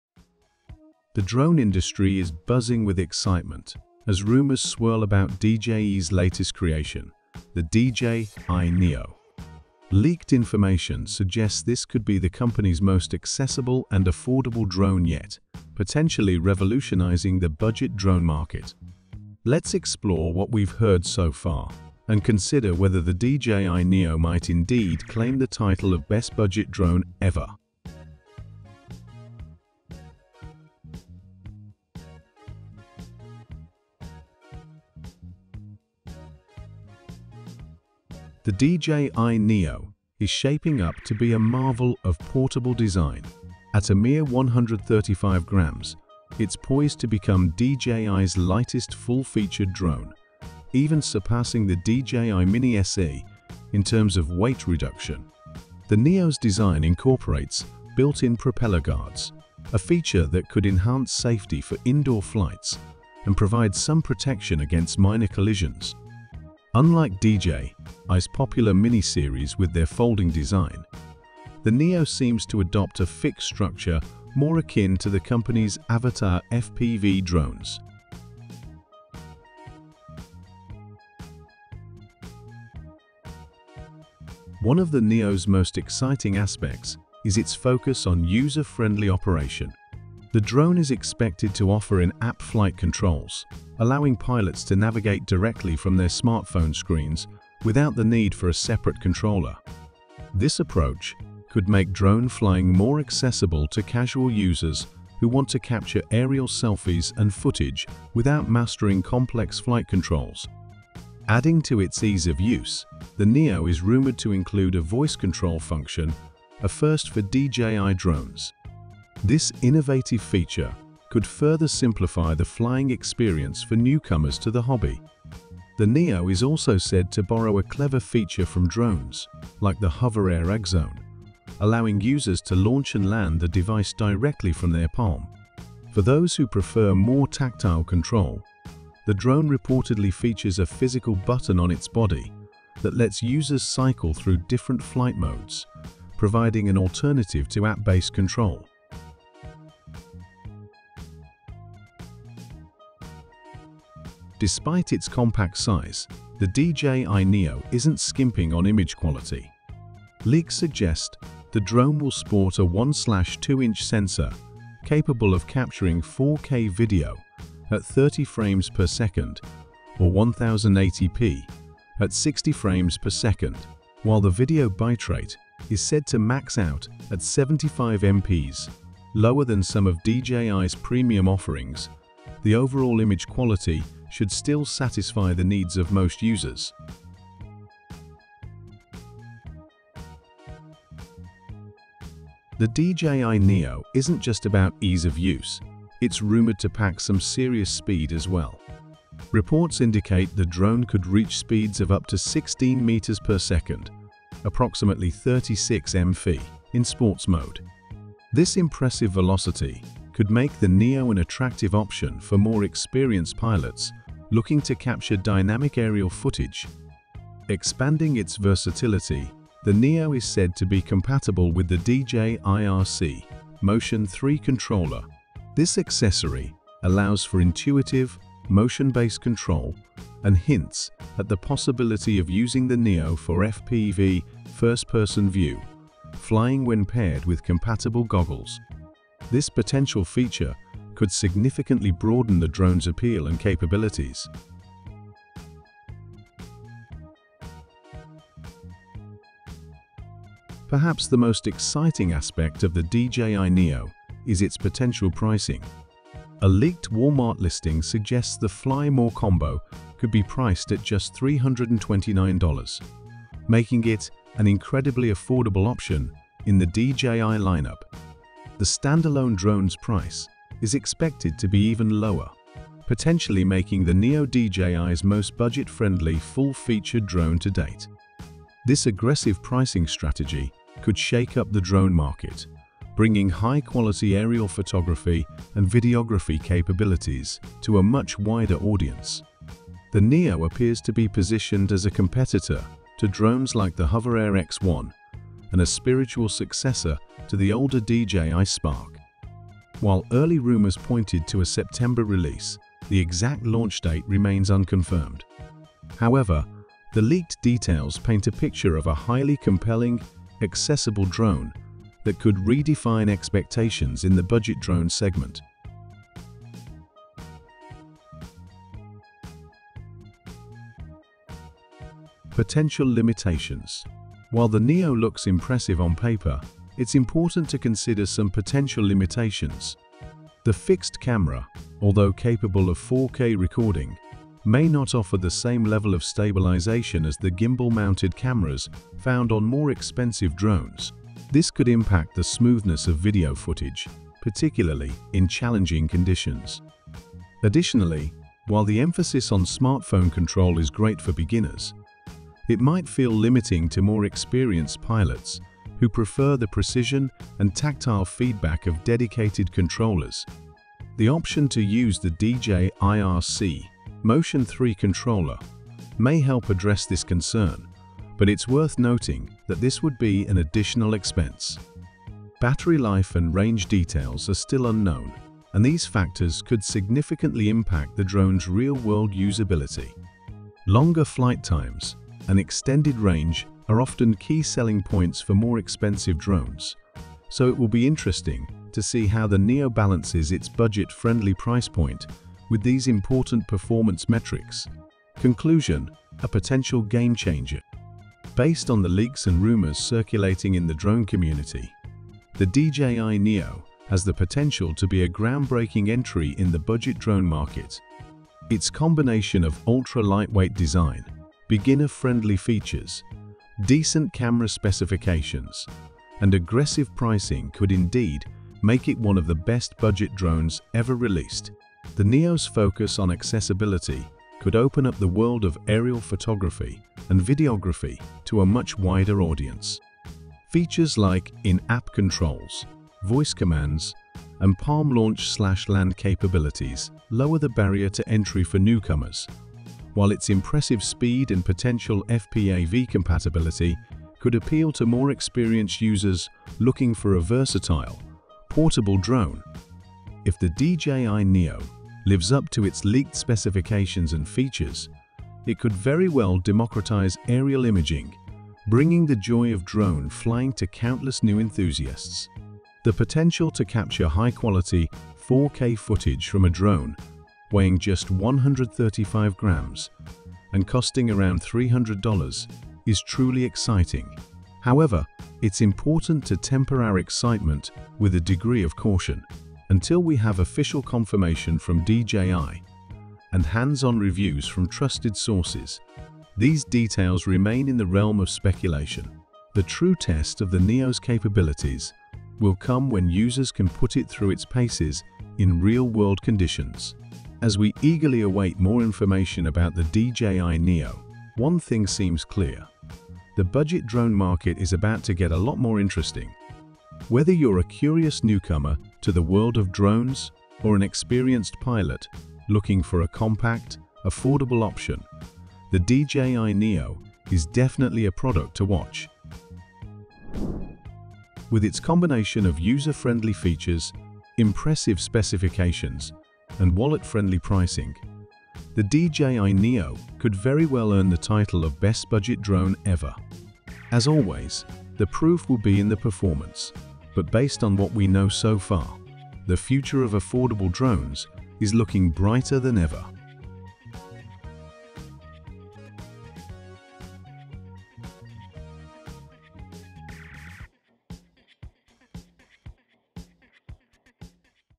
DJI Neo Final Leaks - Release This Week: Great Drone Ever!
DJI, the consumer drone industry leader, is set to revolutionize the market with the DJI Neo. Leaked info suggests the DJI Neo could be their most beginner-friendly and affordable drone yet, potentially redefining budget options. This video explores the rumored features of the DJI Neo, which may offer unprecedented accessibility and value. From compact design to user-friendly controls, the DJI Neo could be a game-changer for novice drone enthusiasts.